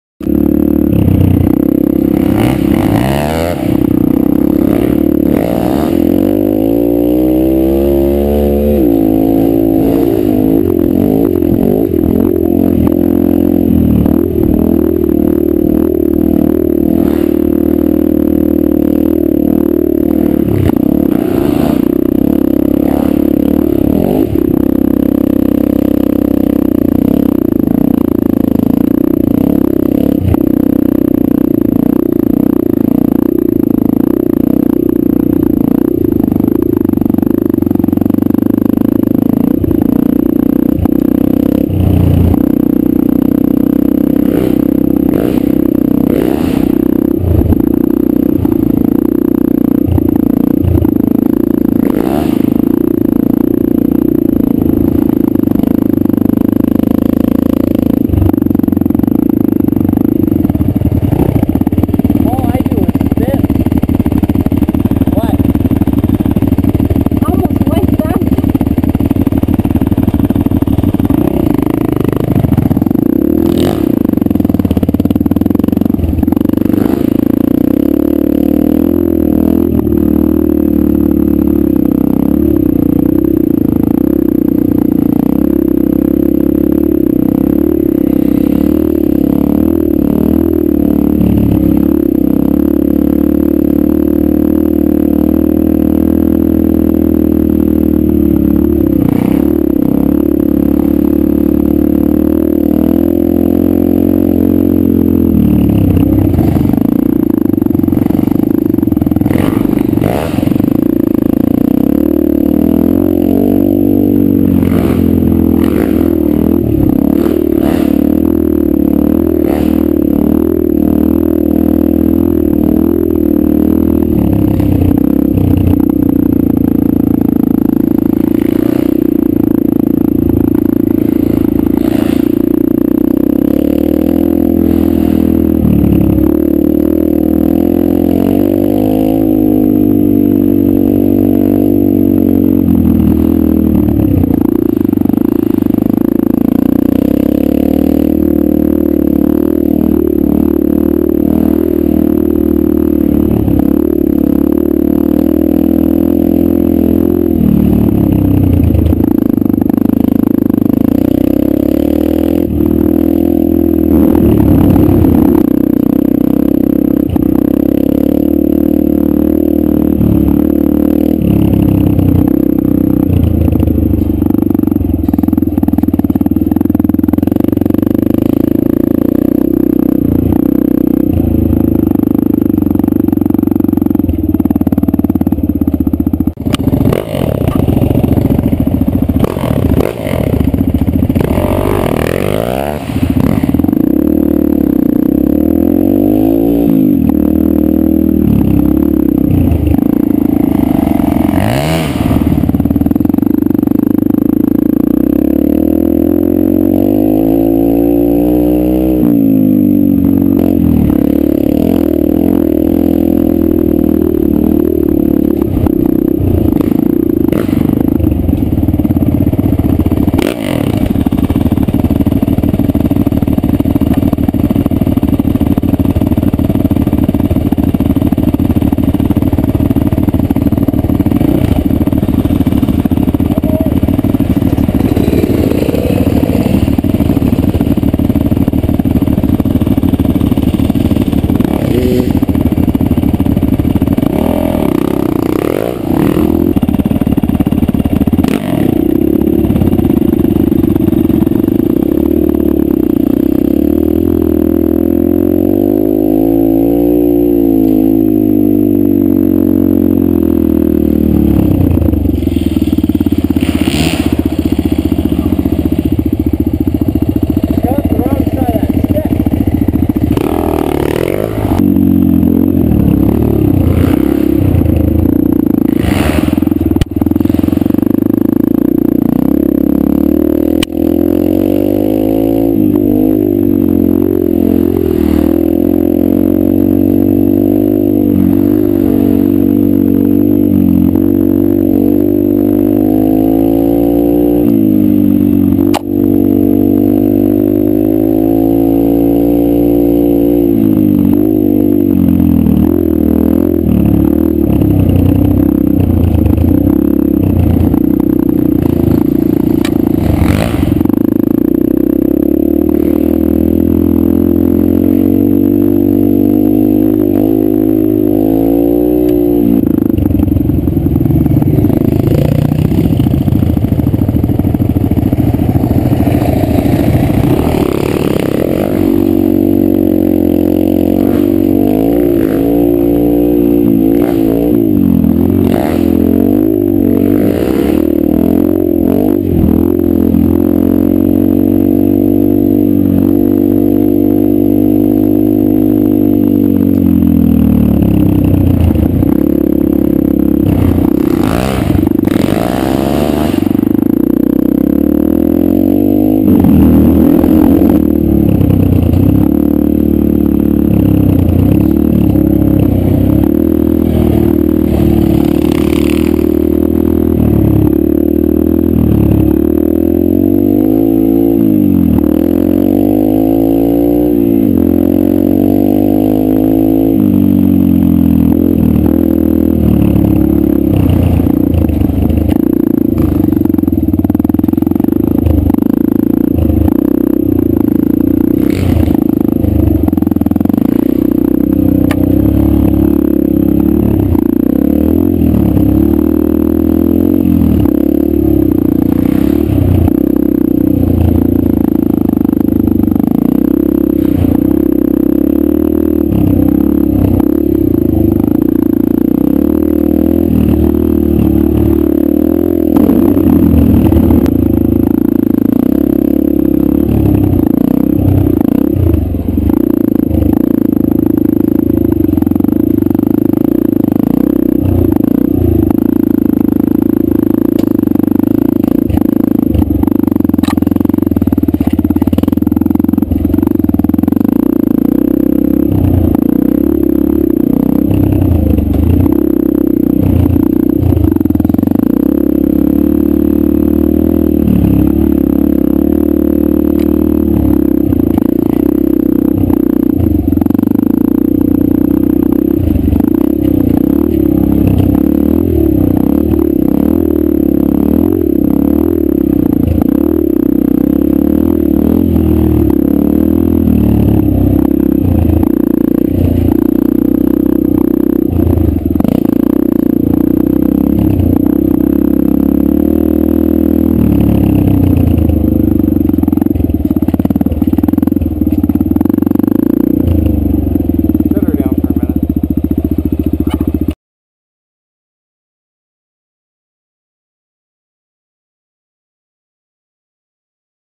Dirt Biking in St. Williams .wmv
Dirt biking in the only section open in St. Williams Forest. The Whoopty Whoops are so close together this year . Hard to get any amount of speed. Turkey Point is closed due too construction in the forest.